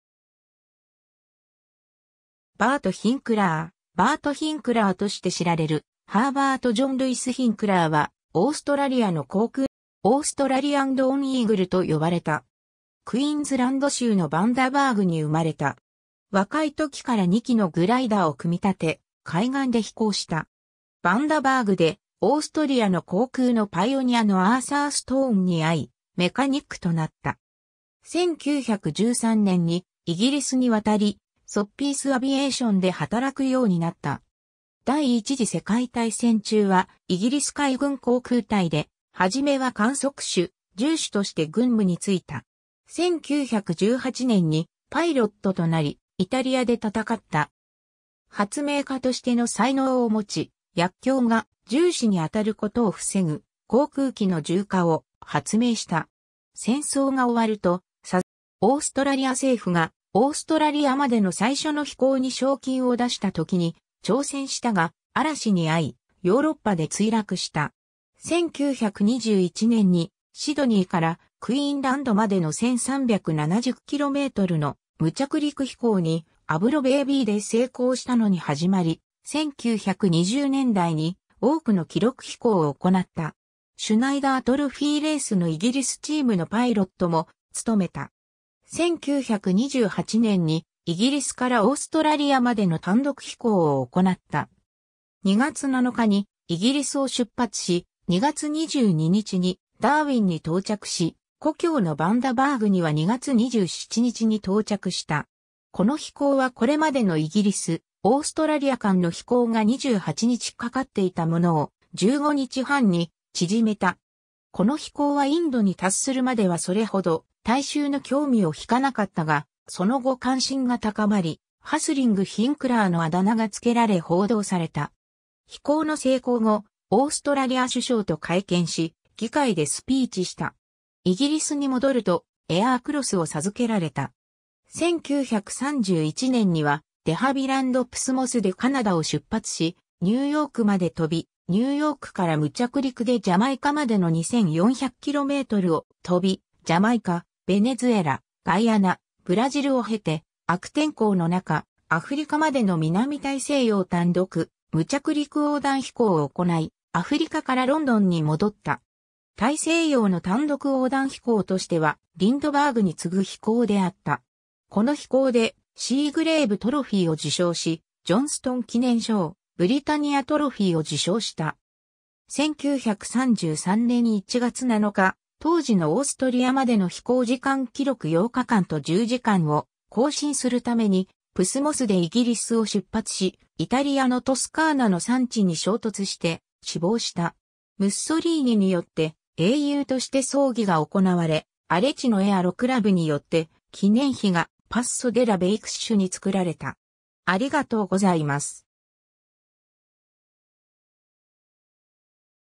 バート・ヒンクラー
Bert Hinkler バート・ヒンクラー(Bert Hinkler)として知られる、ハーバート・ジョン・ルイス・ヒンクラー（Herbert John Louis Hinkler、1892年12月8日 – 1933年1月7日）はオーストラリアの航空のパイオニアで、イギリスからオーストラリアまでの初の単独飛行を行い、南大西洋の単独横断飛行をおこなった。
 "Australian Lone Eagle"と呼ばれた。
 クィーンズランド州のバンダバーグ(Bundaberg)に生まれた。
若いときから2機のグライダーを組み立て、海岸で飛行した。
バンダバーグで、オーストリアの航空のパイオニアのアーサー・ストーン（ARTHUR B. STONE、別名 A.B "Wizard" Stone）に会い、メカニックとなった。
1913年にイギリスに渡り、ソッピース・アビエーションで働くようになった。
 第一次世界大戦中はイギリス海軍航空隊で、初めは観測手、銃手として軍務についた。
1918年にパイロットとなり、イタリアで戦った。
発明家としての才能を持ち、薬莢が銃士に当たることを防ぐ航空機の銃架を発明した。
 戦争が終わると、サザンプトンのアブロ社のテストパイロットとなった。
オーストラリア政府が、オーストラリアまでの最初の飛行に賞金を出した時に、挑戦したが、嵐に遭いヨーロッパで墜落した。
 1921年にシドニーからクィーンランドまでの1,370kmの無着陸飛行にアブロ ベービーで成功したのに始まり、1920年代に多くの記録飛行を行った。
シュナイダー・トロフィー・レースのイギリスチームのパイロットも勤めた。
 1928年にイギリスからオーストラリアまでの単独飛行を行った。
2月7日にイギリスを出発し、2月22日にダーウィンに到着し、故郷のバンダバーグには2月27日に到着した。
この飛行はこれまでのイギリス、オーストラリア間の飛行が28日かかっていたものを、15日半に縮めた。
この飛行はインドに達するまではそれほど大衆の興味をひかなかったが、その後関心が高まり、"Hustling Hinkler"のあだ名がつけられ報道された。
飛行の成功後、オーストラリア首相と会見し、議会でスピーチした。
イギリスに戻るとAir Crossを授けられた。
 1931年にはデ・ハビランド プス・モスでカナダを出発し、ニューヨークまで飛び、ニューヨークから無着陸でジャマイカまでの2,400kmを飛び、ジャマイカ、ヴェネズエラ、ガイアナ、ブラジルを経て、悪天候の中、アフリカまでの南大西洋単独無着陸横断飛行を行ない、アフリカからロンドンに戻った。
大西洋の単独横断飛行としては、リンドバーグに次ぐ飛行であった。
この飛行で、シーグレーブ・トロフィーを受賞し、ジョンストン記念賞(Johnston memorial prize)、ブリタニア・トロフィーを受賞した。
 1933年1月7日、当時のオーストリアまでの飛行時間記録8日間と10時間を更新するために、プスモスでイギリスを出発し、イタリアのトスカーナの山地に衝突して、死亡した。
ムッソリーニによって英雄として葬儀が行われ、Aretino Aero Clubによって記念碑がPasso Della Vaccheに造られた。